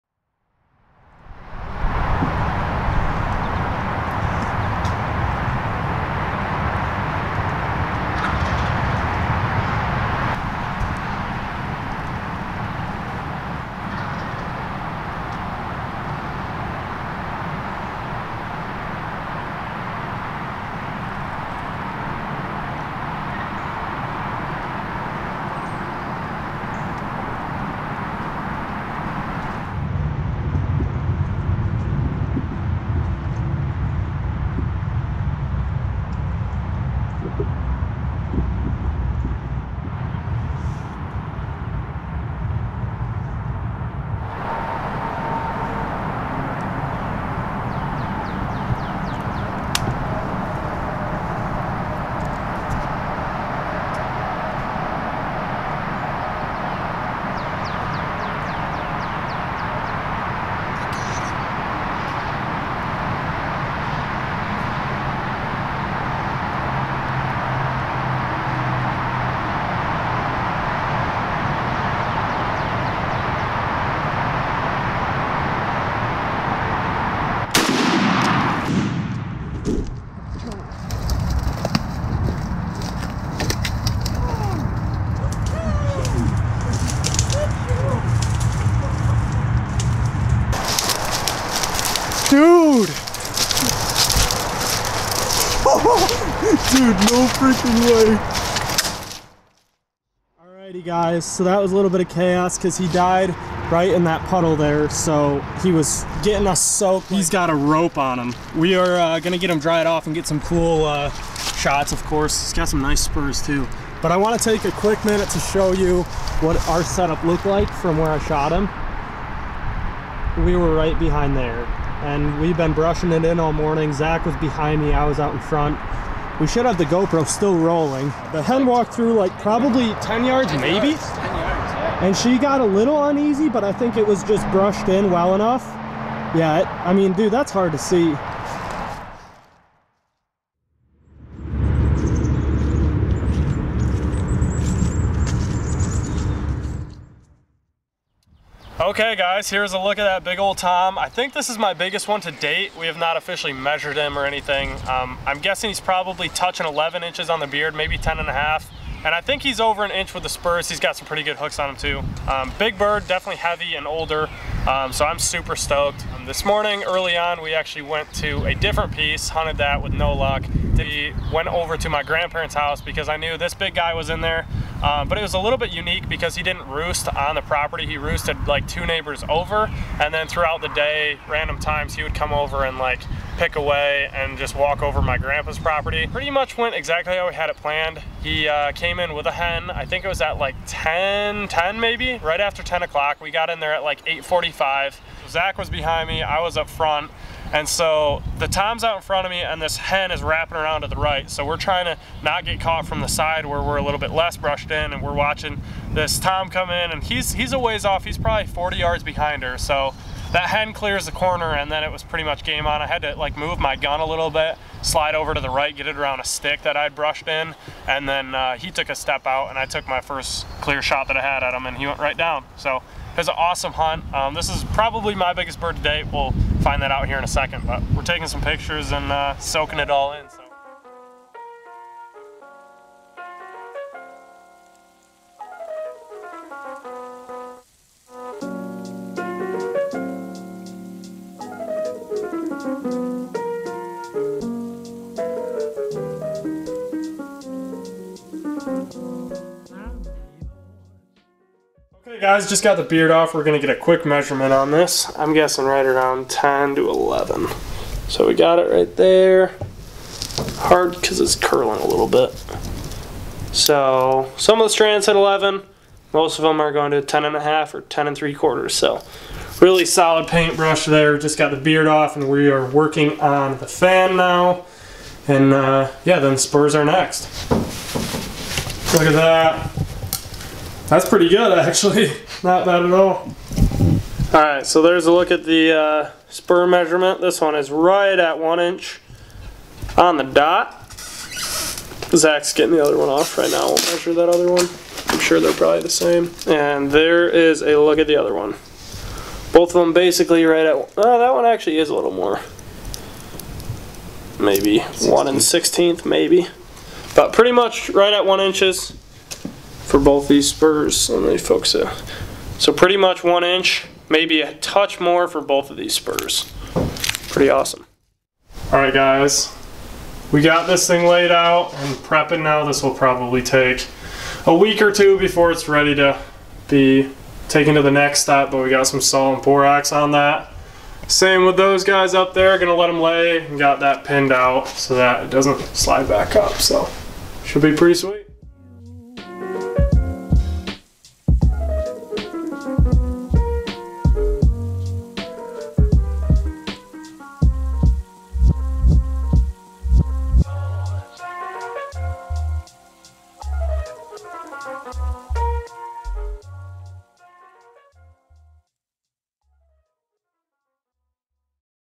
My Biggest Turkey to Date! 2022 Michigan Turkey Season!
Make sure to stay on the lookout for a lot more awesome hunting and fishing videos to drop over the next few weeks. God bless and stay obsessed!

Main Instagram: kyle_kunst22
Fishing insta: reel_obsession_fishing